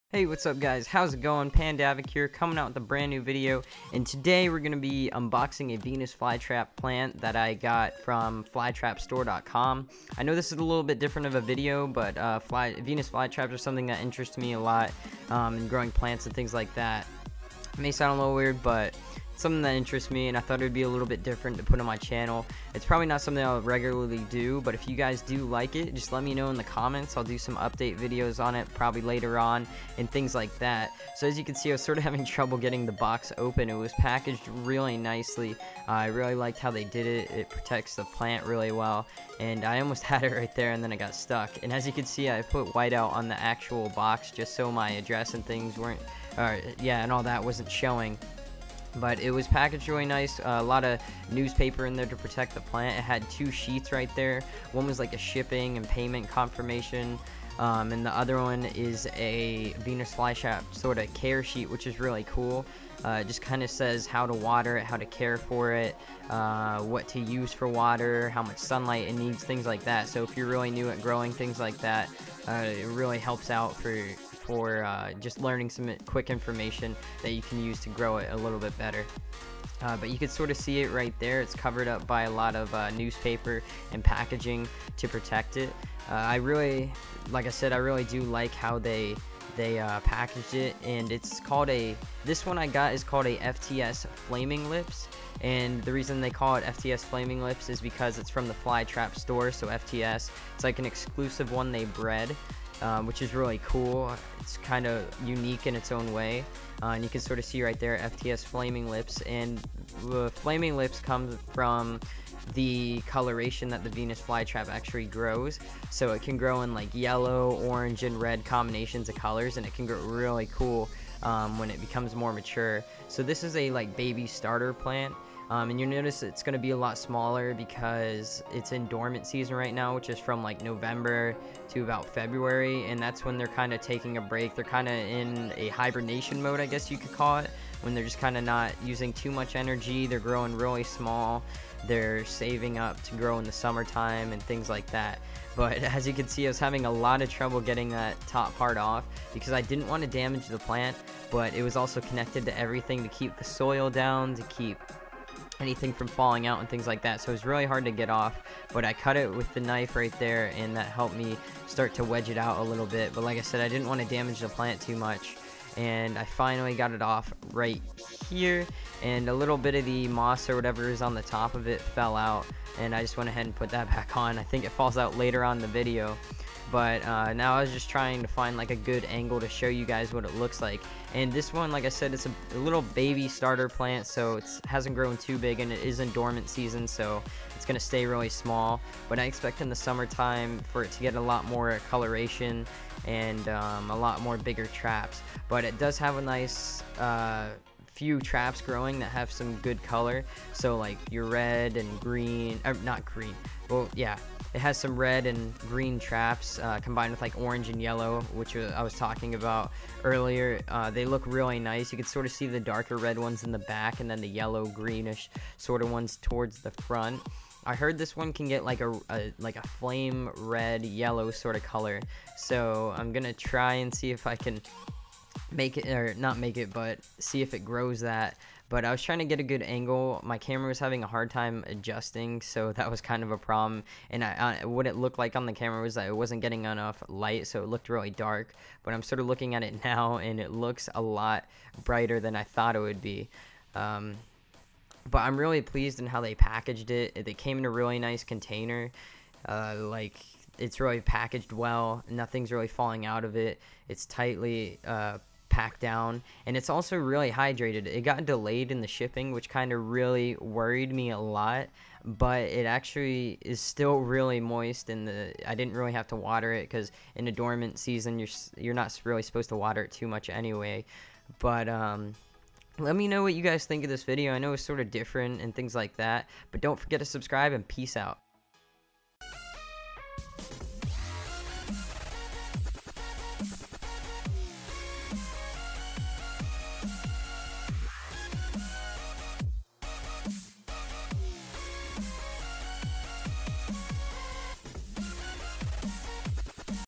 Venus Fly Trap Unboxing! FTS Flaming Lips
Hey guys just me unboxing a Venus Fly Trap I bought from flytrapshop.com. Check them out!
Let me know if you guys enjoyed this and I will try to do this more often.